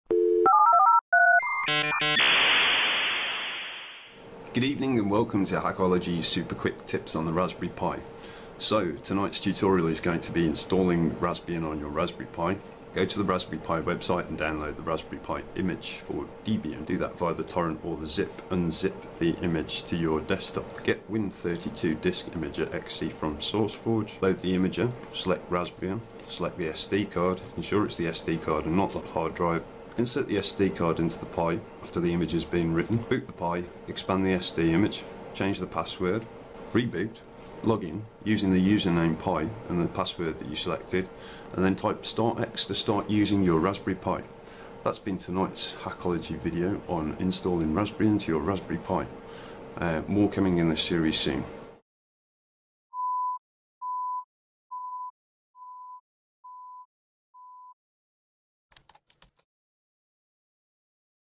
Hakology - How to install raspbian on the raspberry pi
In this video we'll be installing raspbian on the raspberry pi.